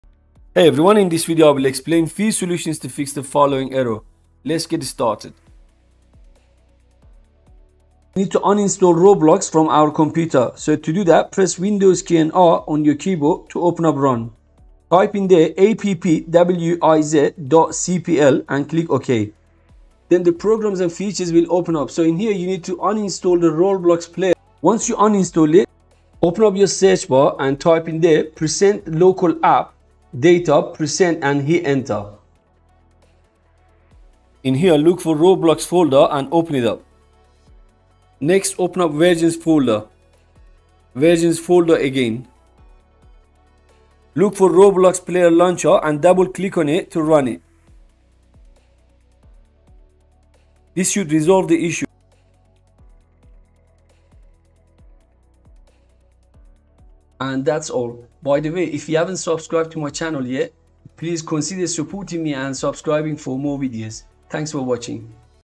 Fixing: "Roblox This File Does Not Have An App Associated With It For Performing This Action"
In this video, I will give you a step-by-step guide to fix the "Roblox error This File Does Not Have An App Associated With It For Performing This Action" error, a simple solution. When trying to play the Roblox game on your Windows 10 or Windows 11 PC.